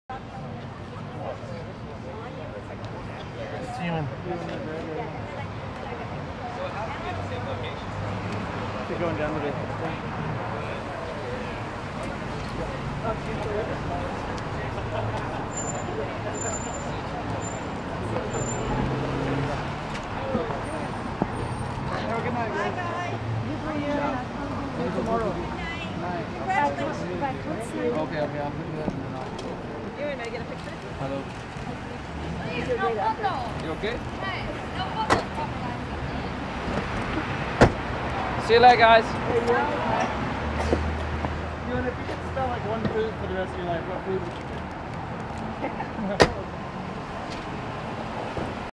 Ewan McGregor and Eve Mavrakis depart at Chateau Marmont ...
Ewan McGregor and Eve Mavrakis depart at Chateau Marmont in West Hollywood, 12/09/12